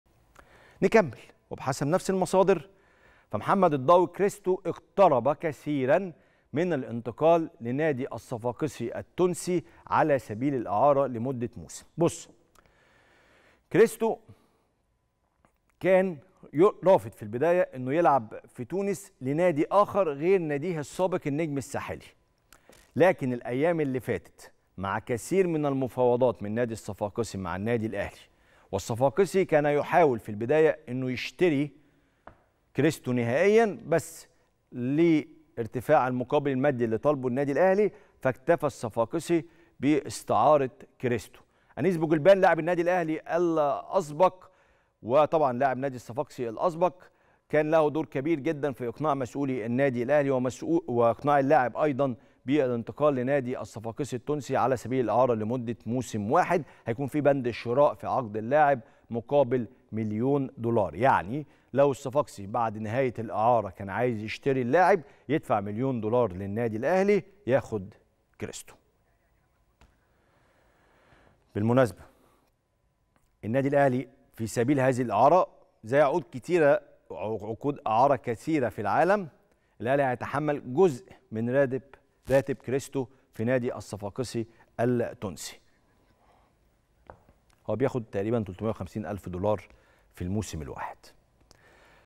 الأهلي يتحمل جزء من راتبه🤔 .. كريم رمزي يكشف اخر تفاصيل مفاوضات الصفاقسي التونسي مع كريستو 🔥💥
انتماء اللاعب هو من عطل الصفقة! والأهلي يتحمل جزء من راتبه🤔 .. كريم رمزي يكشف اخر تفاصيل مفاوضات الصفاقسي التونسي مع كريستو 🔥💥